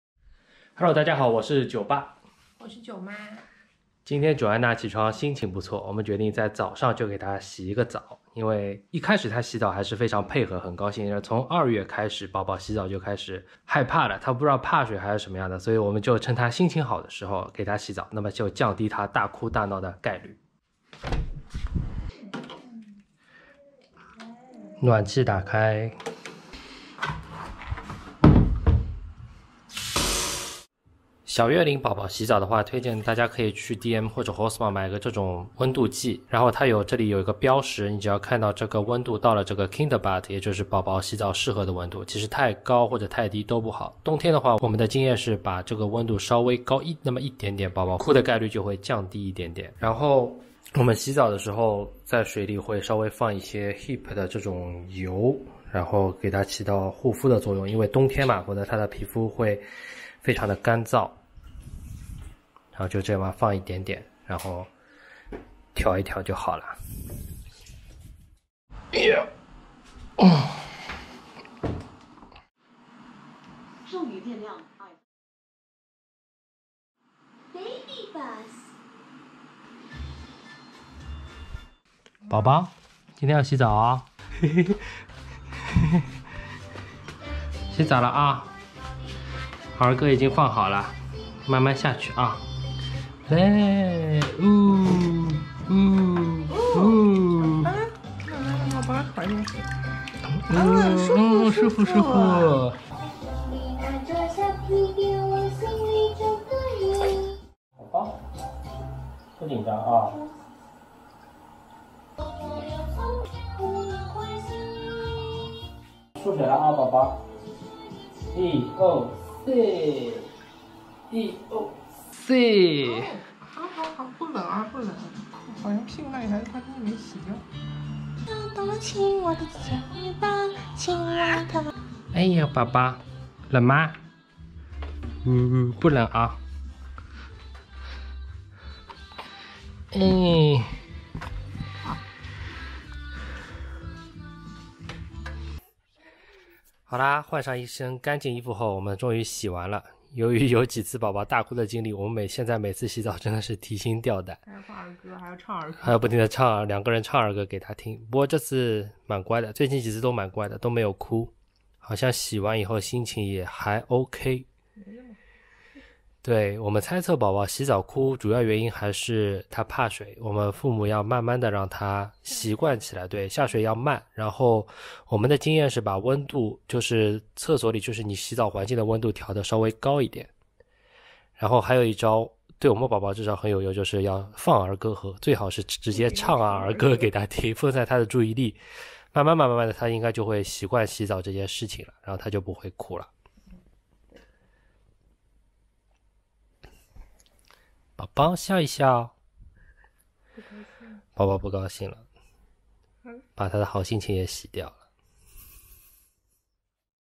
[德国生活Vlog] 小月龄宝宝洗澡大作战 (Joanna的成长日记03)
Joanna两个月大之前很喜欢洗澡，但是过了两个月洗澡就经常大哭，每次洗澡都像打仗一样。我们摸索出了一点点给宝宝洗澡的经验与大家分享